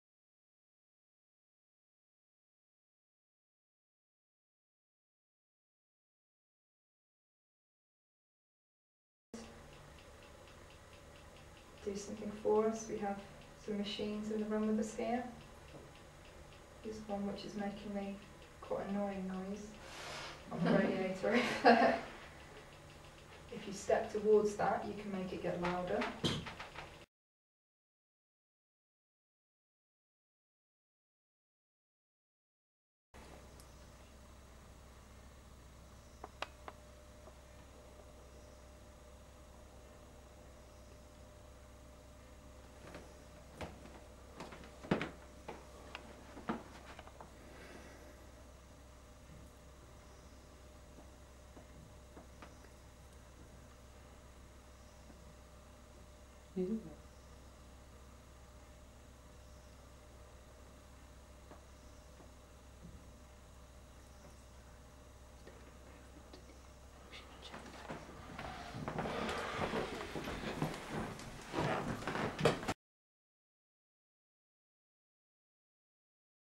Paranormal Interactive Footage: York Brewery
Footage of physical phenomena, interaction and response during an investigation at the York Brewery.